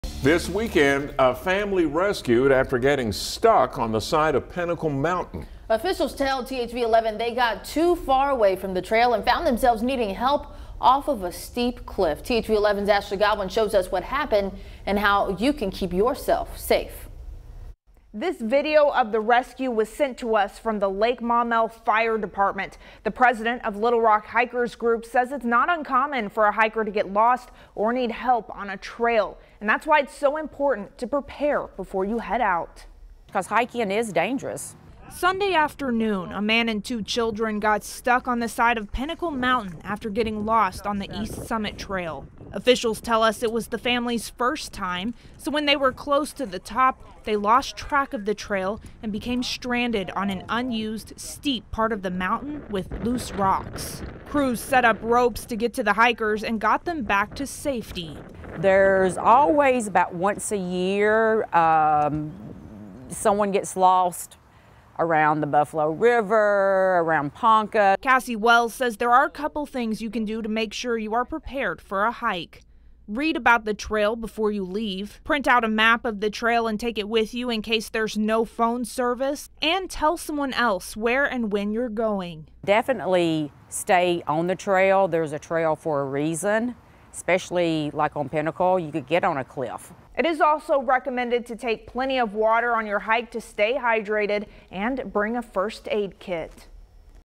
Family gets rescued from Pinnacle Mountain
Over the weekend, one family found themselves too far away from the trail and needed to be rescued from the side of Pinnacle Mountain.